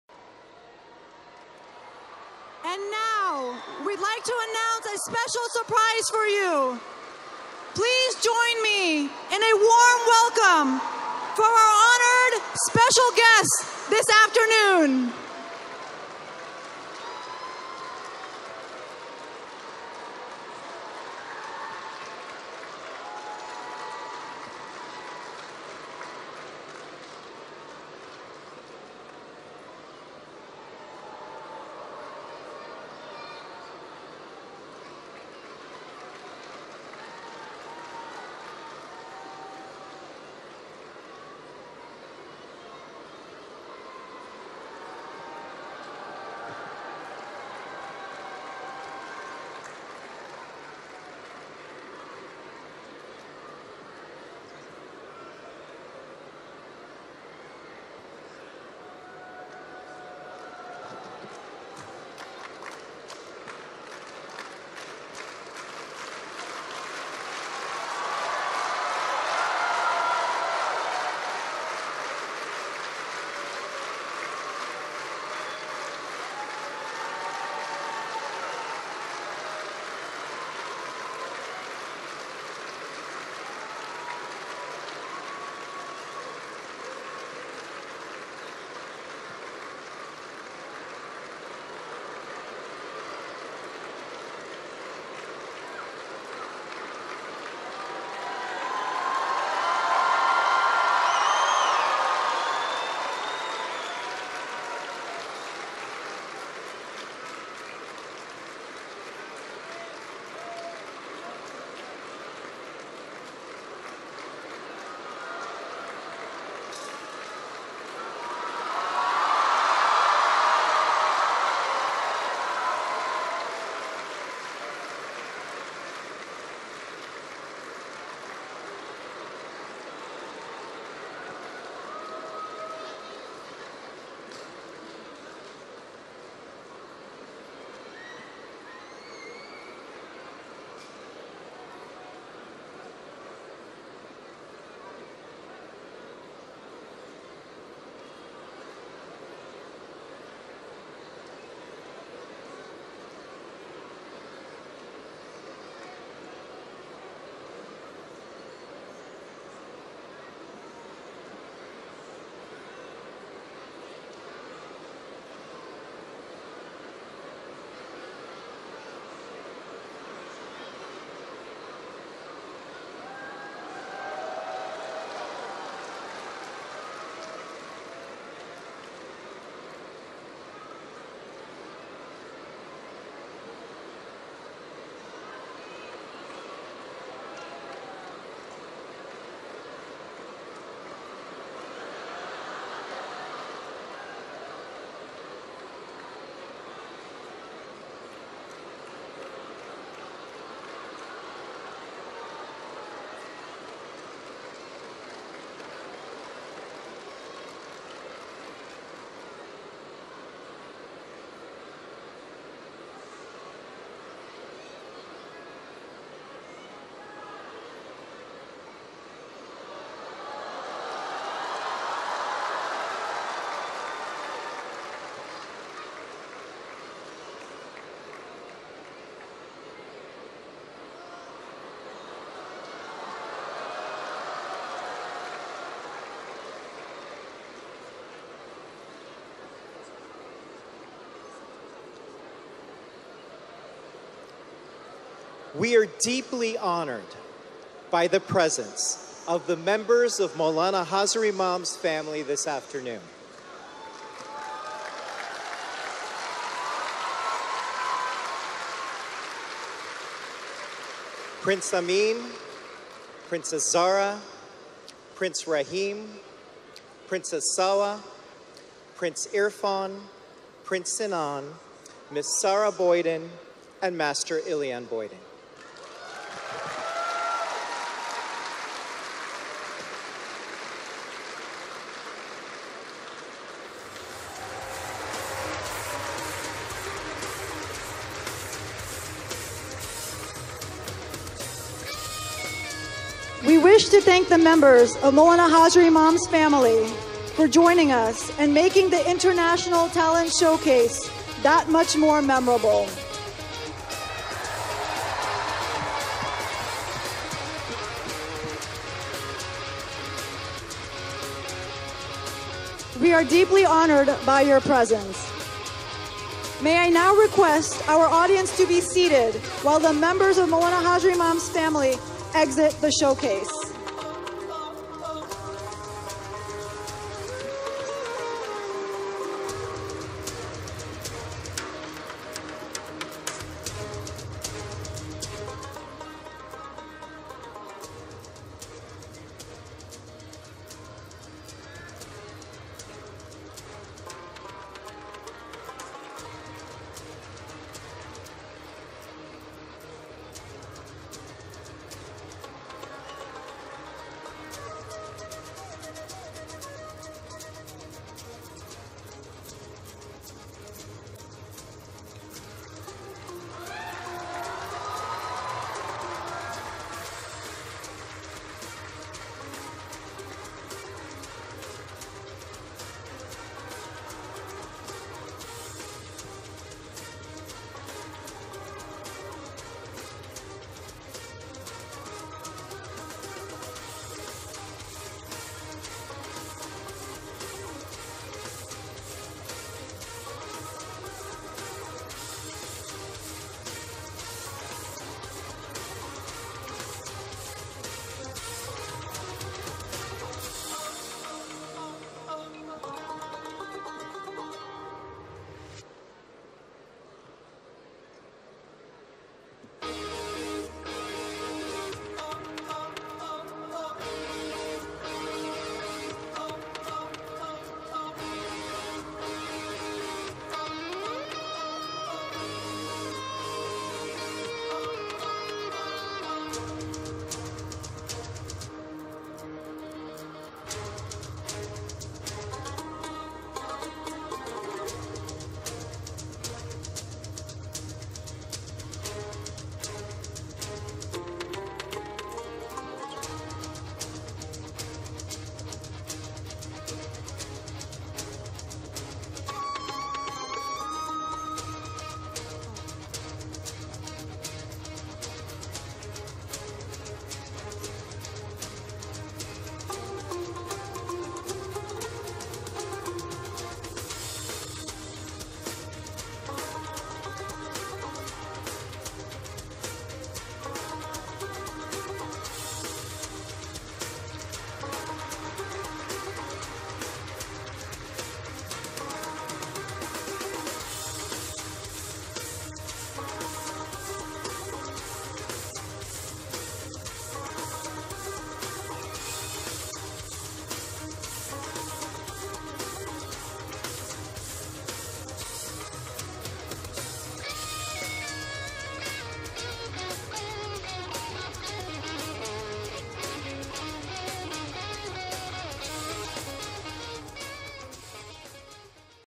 Aga Khan Diamond Jubilee Lisbon, Portugal - Day 2 Noorani Family Visits Venue
During the Diamond Jubilee Celebrations in Lisbon, Portugal, held at the Parque das Nações venue, The Noorani Family consisting of Prince Rahim Aga Khan and Princess Salwa Aga Khan along with sons Prince Irfan and Prince Sinan, joined by Prince Amyn Muhammad Aga Khan, Princess Zahra Aga Khan along with children Sara Boyden & Master Iliyan Boyden, who graciously attended the Jubilee Arts Talent Showcase - "Wonder" after visiting the Rays of Light exhibition and the International Art Exhibition. This was an incredible moment for the thousands of members of the Jamat who were at the venue site who captured glimpses of the Noorani Family as they passed through the venue amidst security as well as those murids who were fortunate to be a part of the Talent Showcase on July 9th, 2018.